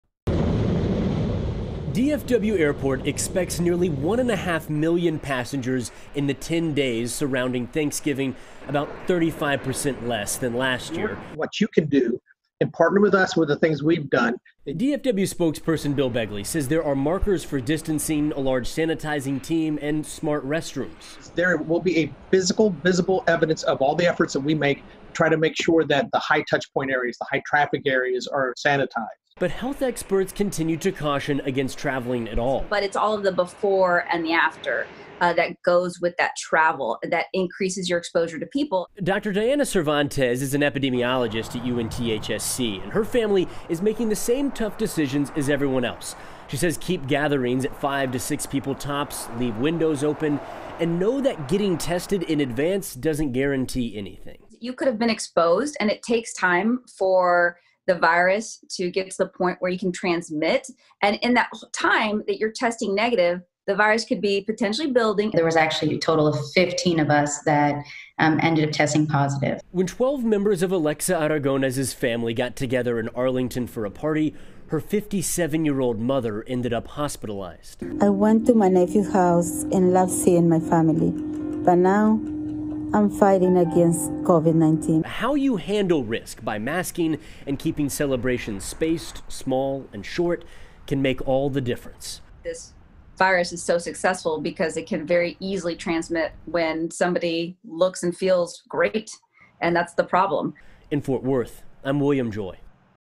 North Texas health experts, airports brace for busy Thanksgiving travel amid pandemic
The CDC advises against traveling for the holiday. There are tips for reducing risk for those who do.